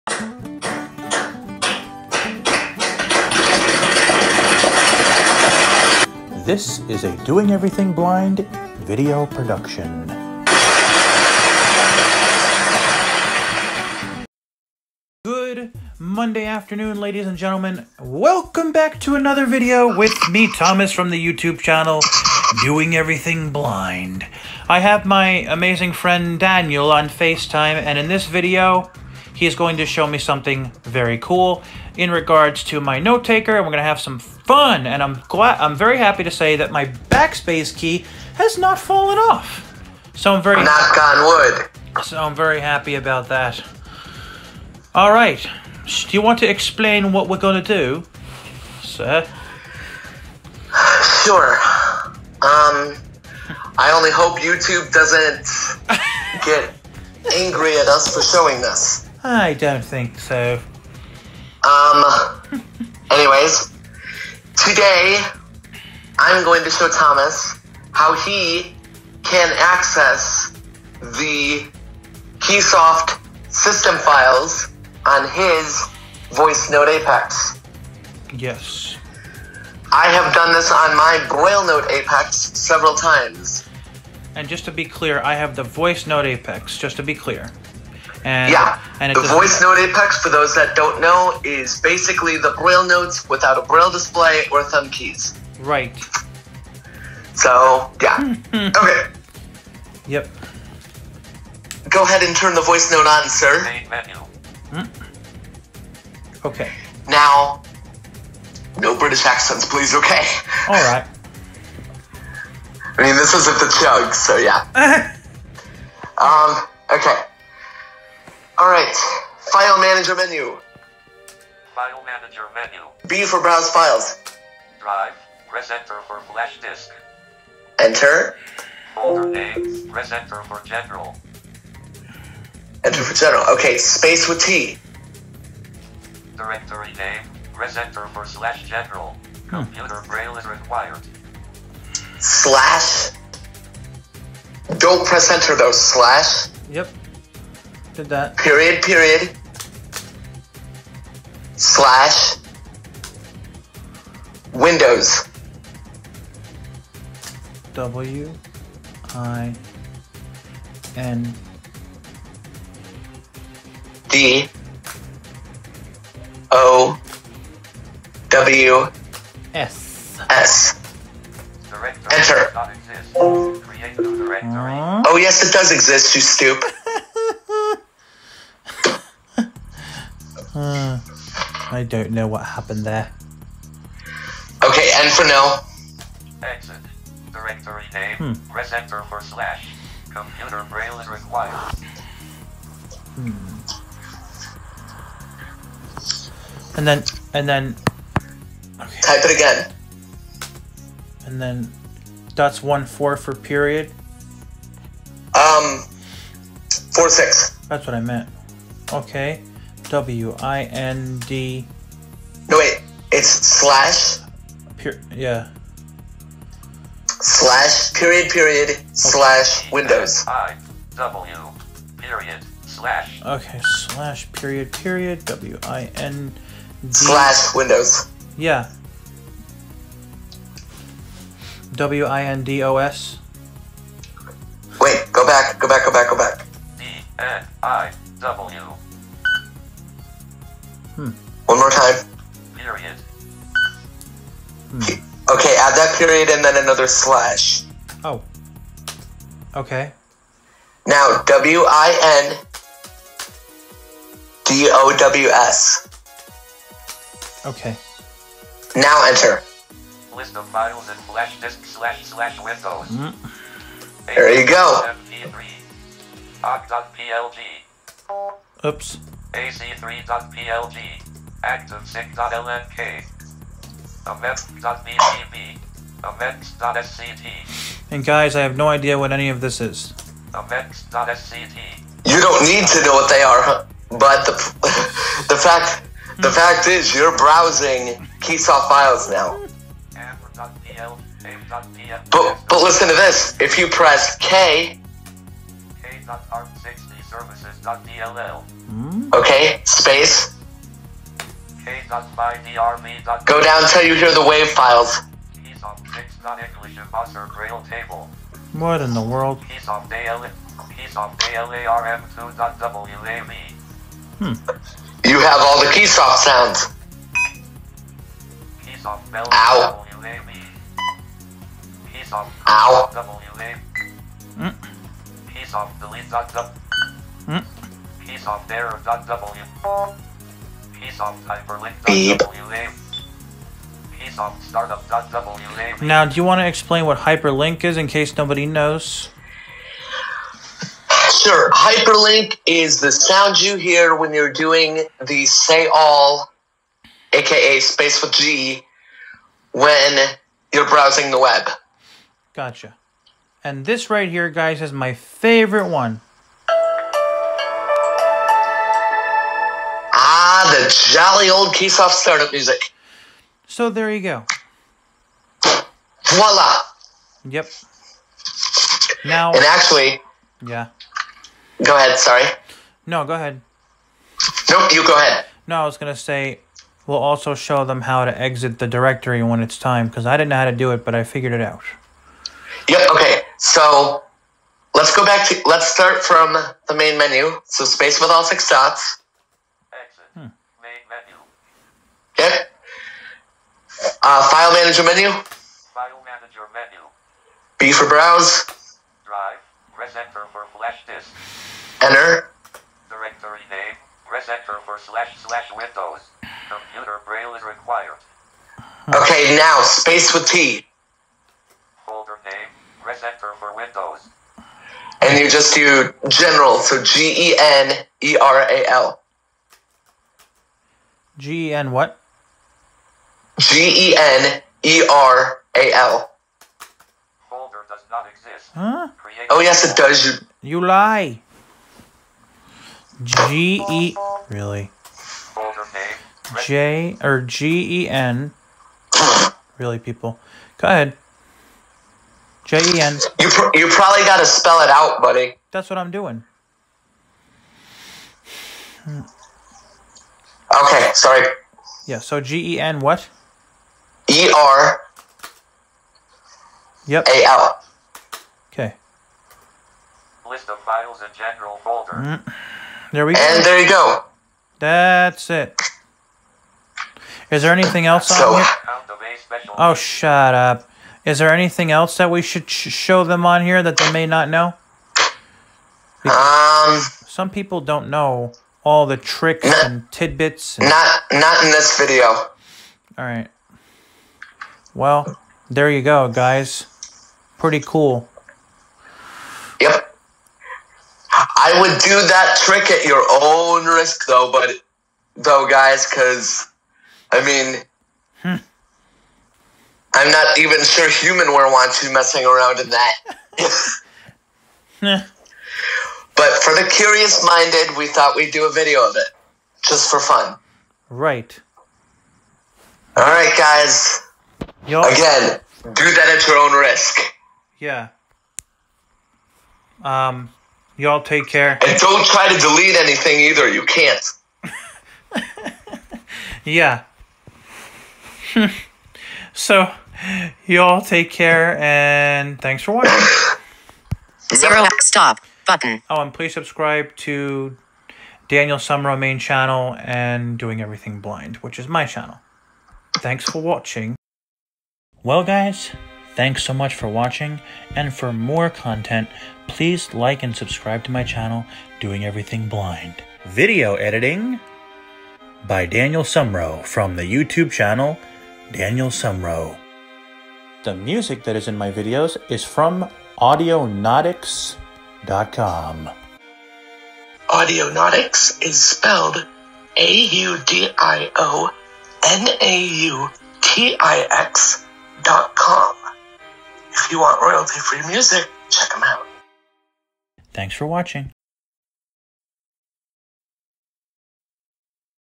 how I access the KeySoft system files on my VoiceNote Apex
Note that computer braille is required, so don't try to use grade 2 braille for typing the directory name.

6. Press enter.

It may take a few minutes to seconds for the device to load the file list because there are so many!

Thank you to Daniel Semro for teaching me this trick.

As he says in this video, use the trick at your own risk. We're not responsible if you mess up your apex.

Instructions:

1. Go to the main menu by pressing space with all 6 dots.
2. Press F to get to the file manager menu.
3. Press B for browse files.
4. At the "folder name?" prompt, press the keystroke space with T to access the directory layer.

5. Once the notetaker speaks/displays "directory name?" followed by "/name" (where name is the name of the folder chosen), type the following:

"/ (slash), .. (period, period) / (another slash), windows"